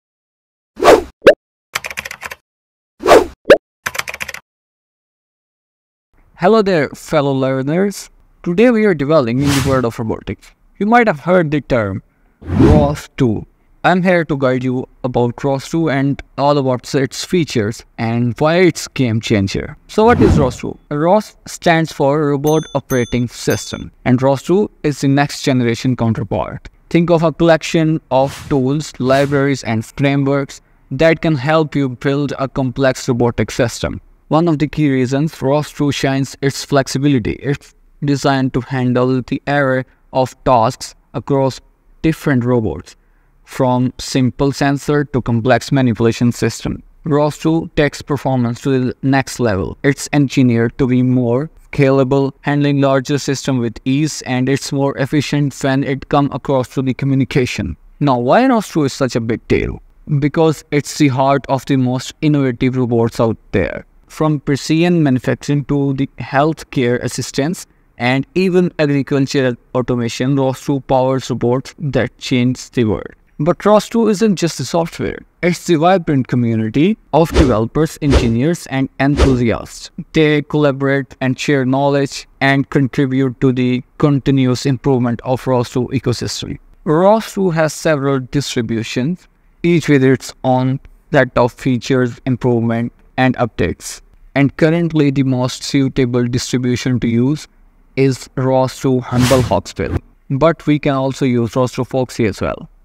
What is ROS? | ROS2 for Beginners and Microros with ESP32 Course
ROS2 for Beginners: Build Your First Robot with ESP32

Welcome to the world of robotics and creativity! Join us on an exciting journey as we introduce you to Lineturtle, a project that combines hands-on learning with the power of ROS2 and MicroROS. Build a drawing robot, master command-line control, and turn your ideas into reality!

Get ready to build, create, and innovate with Lineturtle!